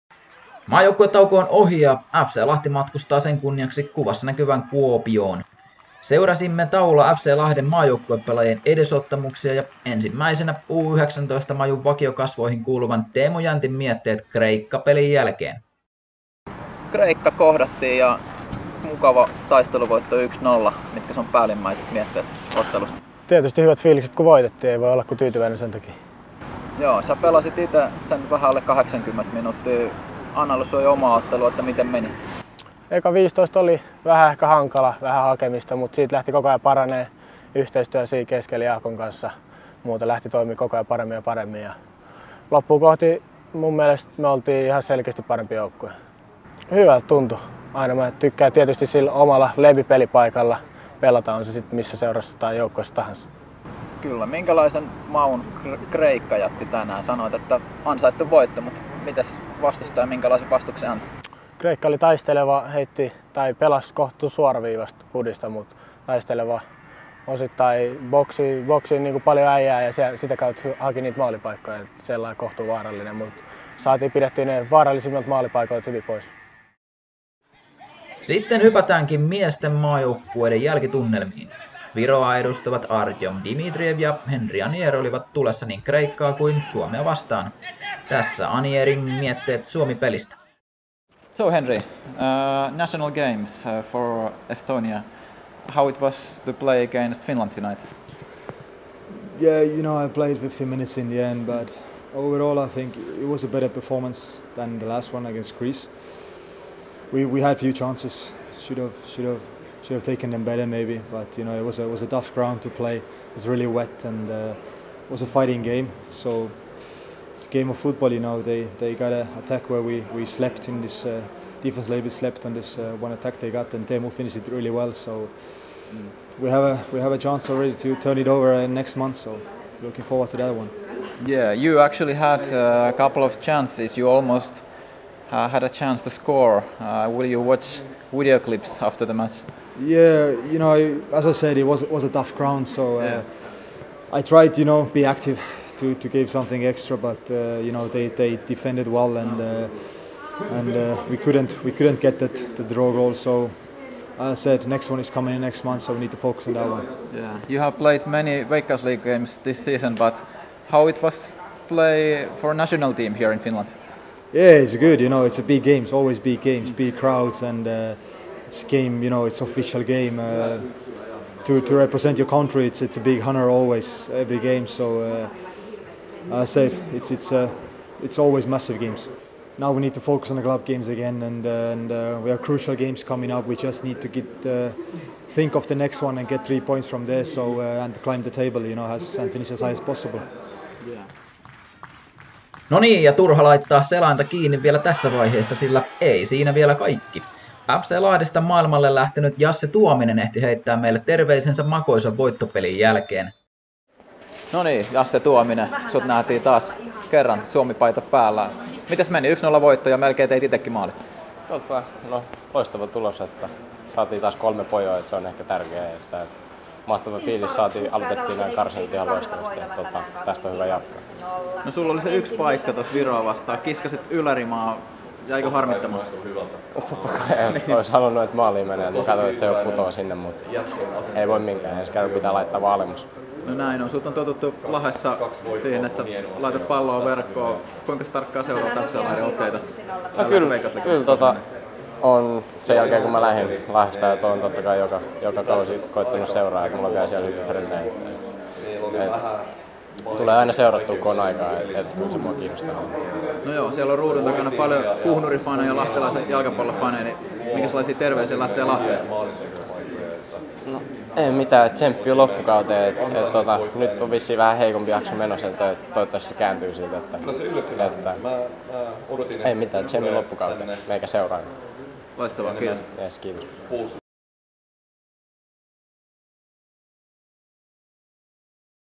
Otteluennakko: KuPS-FC Lahti: Tunnelmia maajoukkuepeleistä
FC Lahti kohtaa KuPS:n Kuopiossa torstaina. Tässä majutauon tunnelmia U19-pelistä Hämeenlinnasta ja A-maajoukkueen ottelusta Turusta. Äänessä Teemu Jäntti ja Henri Anier. Ja ehtipä Jasse Tuominenkin heittää meille terveiset.